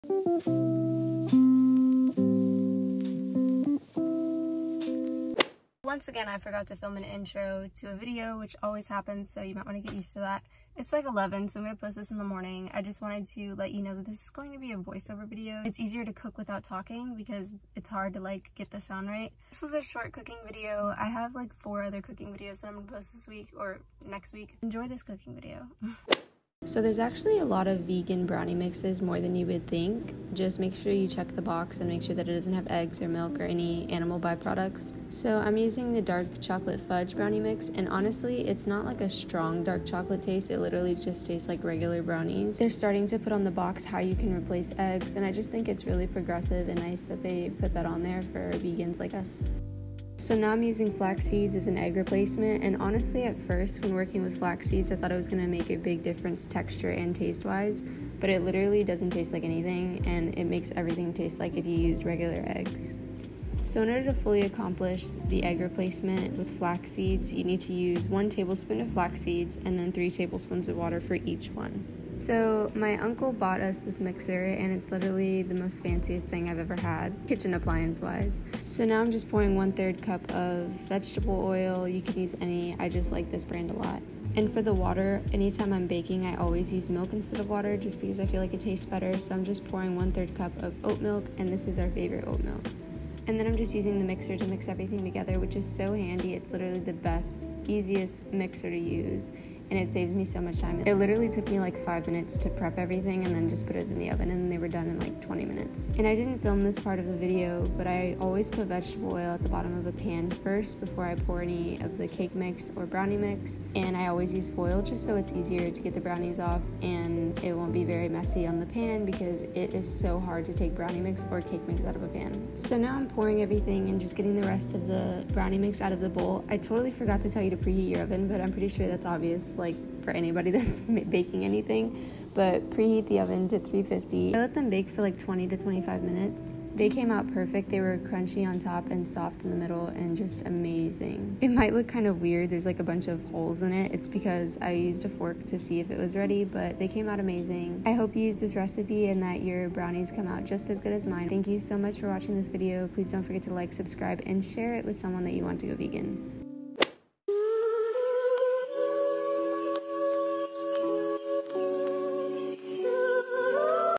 How to make vegan brownies (:
Anothaaaa cooking video ! Well .. baking but you get it :p I had to make vegan brownies because EVERY vegan loves brownies. Love youuu

Ingredients & measurements :
- Preheat your oven to 350 (:
- Duncan Hines Dark Chocolate Fudge Brownie mix
- 1 Tablespoon of flaxseed ( Use 3 tablespoons of water for each tablespoon of flaxseeds )
- 1/3 cup of vegetable oil
- 1/3 cup of oat milk
- Mixxx (:
_ Get a pan, put foil over it, put vegetable oil on it ( a little bit )
- Pour the mix & put it in the oven for 20-25 minutes or until you feel it's ready